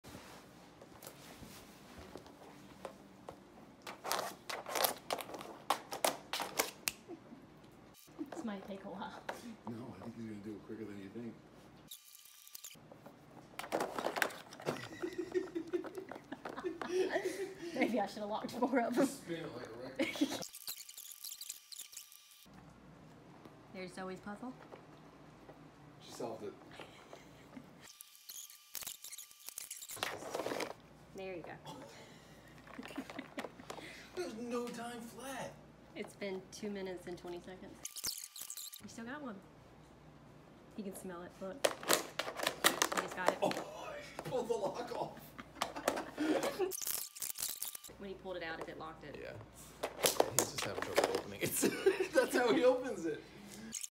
Brilliant Husky Solves Advanced Puzzle Toy - Cute and Funny
Watch how our quickly our Husky (Steele) solves his advanced puzzle toy.  He is so smart!